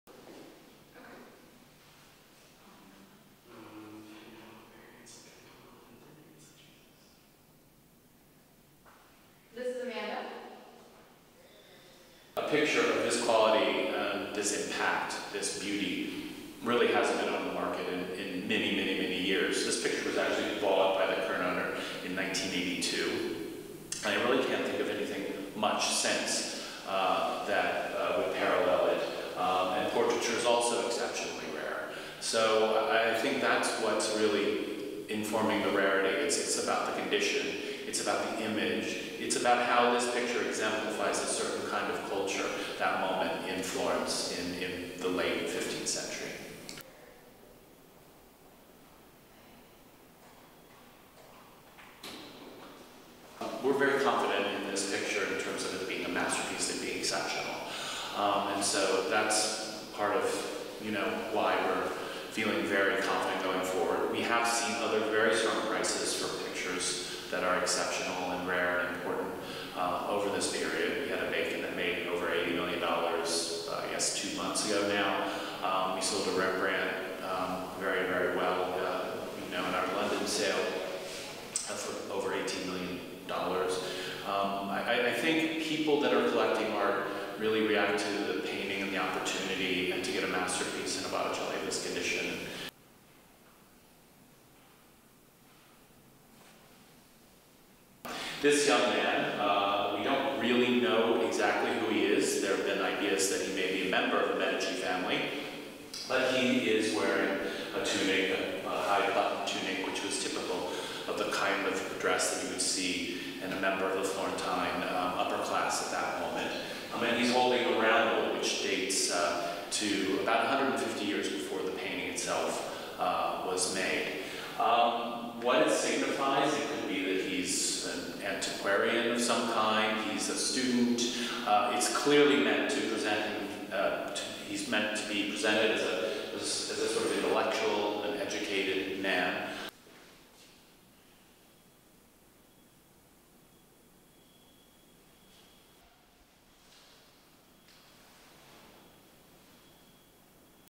Portrait by Renaissance master expected to soar past $80M
(24 Sep 2020) PORTRAIT BY RENAISSANCE MASTER EXPECTED TO SOAR PAST $80M
An enigmatic painting from Renaissance master Sandro Botticelli will go on auction next year and art watchers will be seeing if it fetches more than its eye-watering $80 million estimate, despite the pandemic.

Botticelli’s 15th-century portrait of a nobleman in “Young Man Holding a Roundel” is the highlight of Sotheby’s Masters Week sale series in New York in January.

Christopher Apostle, who has for more than three decades handled Old Masters and is now head of the division, says "a picture of his quality and this impact, this beauty, really hasn't been on the market in many years."

Opportunities to acquire a Botticelli - the artist behind such masterpieces as “Primavera” and “The Birth of Venus” - are very rare and Apostle is confident the painting will sell for a sum close to its $80 million estimate.
If it reaches those dizzying heights, it would represent a windfall for the present owner. The painting was last acquired at auction in 1982 for £810,000 (or just over $1 million today).

The painting - believed to have been executed in the late 1470s or early 1480s - actually represents two art works. Botticelli painted the noble sitter but the roundel - a circular disc used as a symbol - depicts a saint and is an original 14th-century work attributed to the Sienese painter Bartolommeo Bulgarini.

Who the young man depicted has been lost to history as well as why he holds the roundel. Some scholars believe he is associated with the ruling House of Medici or another powerful family in Florence.

Apostle says some things can be inferred: The young man's hair is long and fashionable for the time. His tunic is buttoned up and restrained, dressed in a republican way.

“It could be that he's an antiquarian of some kind. He's a student. It's clearly meant to present, he's meant to be presented as a sort of intellectual and educated man."

In the past 50 years, the painting has spent extended periods on loan at the National Gallery in London, the Metropolitan Museum of Art in New York and the National Gallery of Art in Washington, D.C.